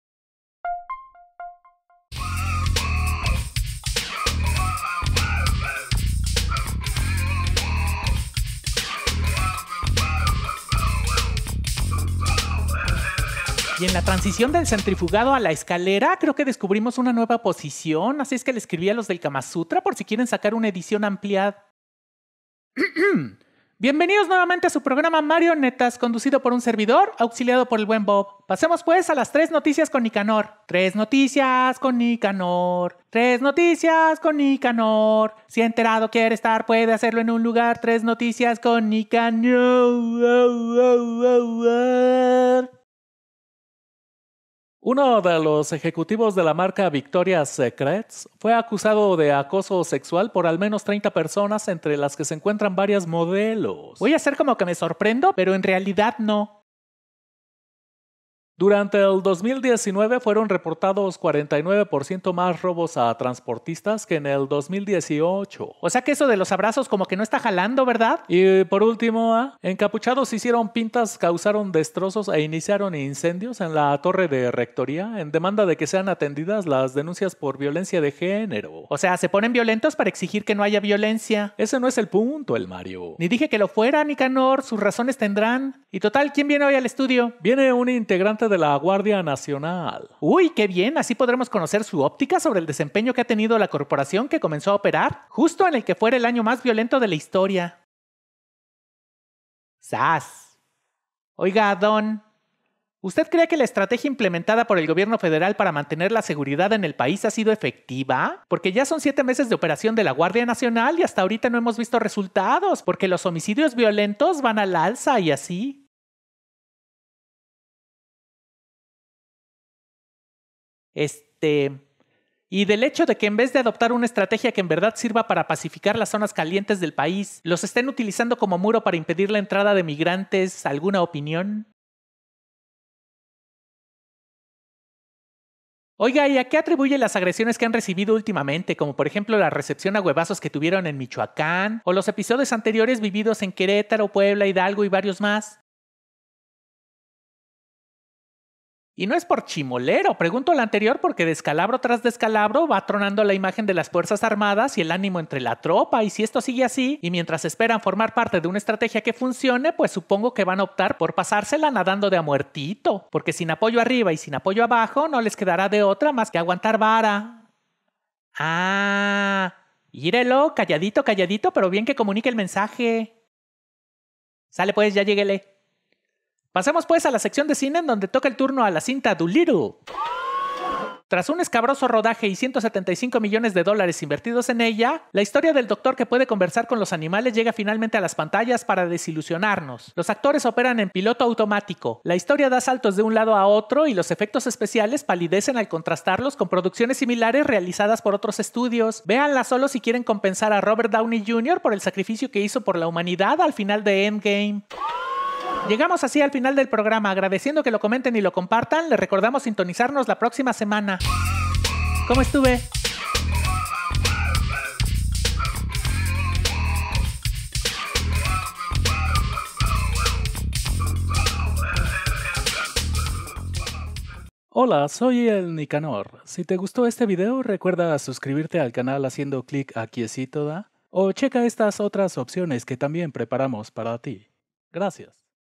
¿Cuál es el futuro de la GN? | Mario Netas
Elemento de GN llega al estudio para platicar sobre situación y futuro de corporación. Además, 3 noticias con Nicanor y en sección de cine: Dolittle.